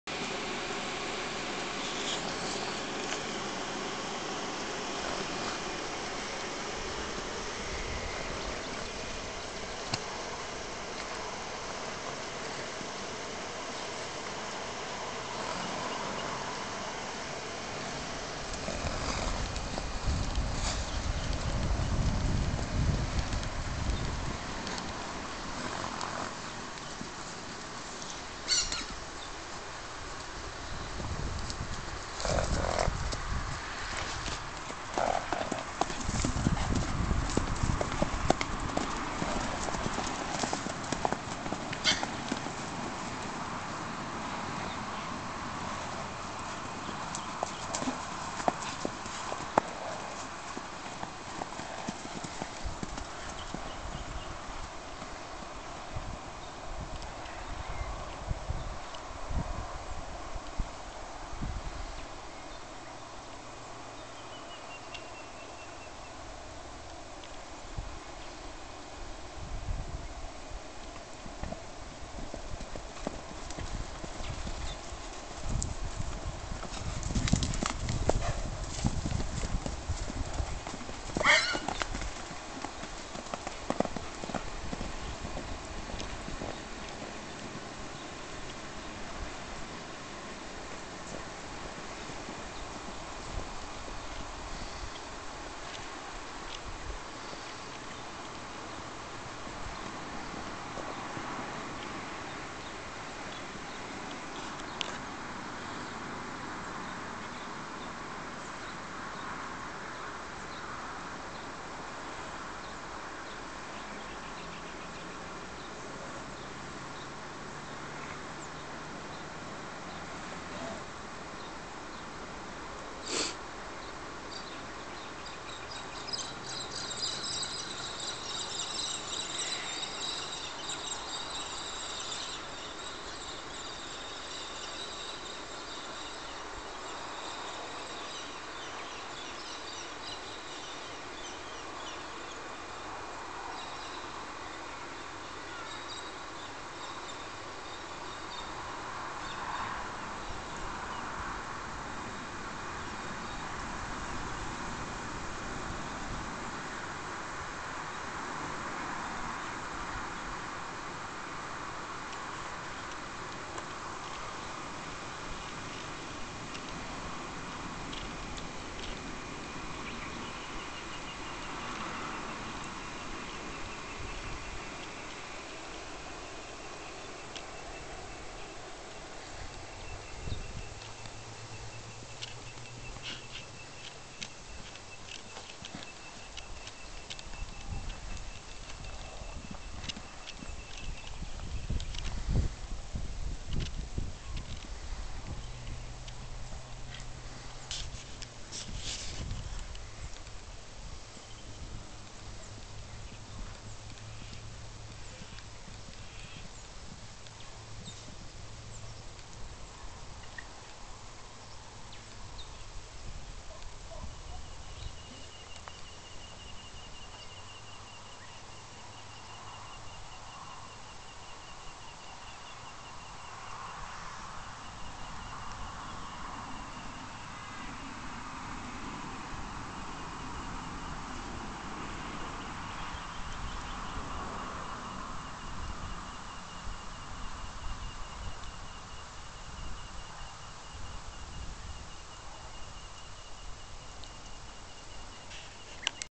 Our horses having a play in the paddock.
The gray horse is new. Reg, Toby and Lily in their first movie!